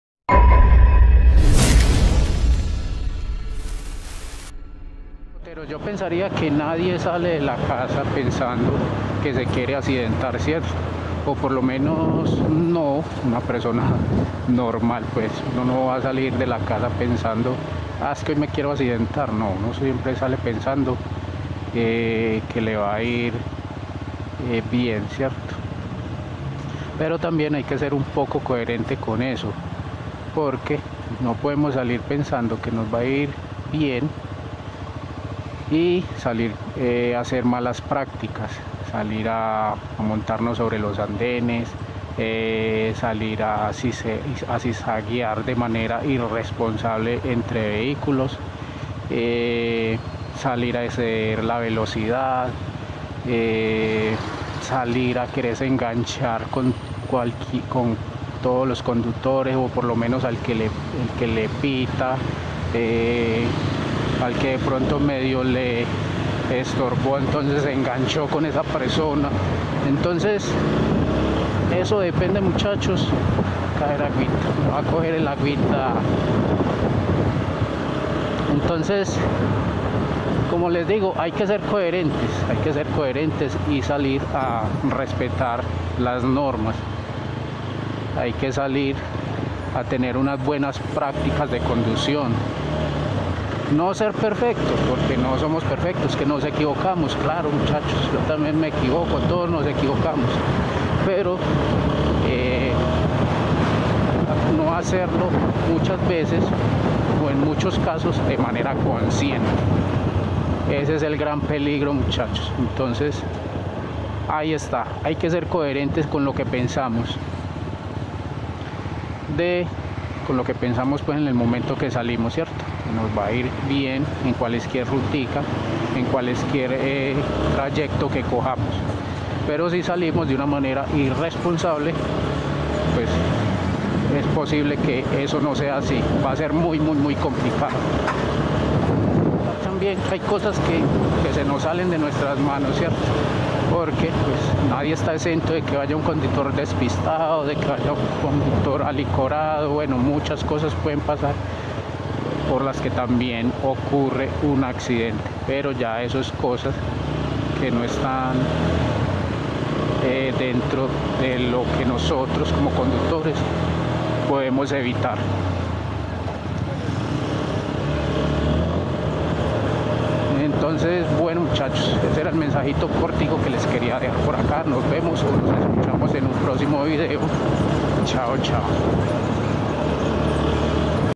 🔴 Porqué La Coherencia Es Tu Mejor Copiloto.
@LUCHO.R.P.M ➡️ Aquí te dejo algunos consejos prácticos para mejorar tu coherencia al conducir:

   ➡️ Evita el uso del celular: El uso del teléfono móvil es una de las principales causas de distracción al conducir.
    No conduzcas bajo los efectos del alcohol o drogas: El consumo de sustancias psicoactivas afecta tus reflejos y tu capacidad de concentración.✔️
   ➡️ Mantén una distancia segura: Deja suficiente espacio entre tu motocicleta y los demás vehículos.✔️
    ➡️ Revisa el estado de tu motocicleta: Asegúrate de que tu motocicleta esté en óptimas condiciones antes de salir a la carretera.✔️
    Utiliza el equipo de protección adecuado: El casco, las chaquetas, los guantes y los pantalones te protegerán en caso de caída.

➡️Recuerda: ¡La seguridad vial es responsabilidad de todos!✔️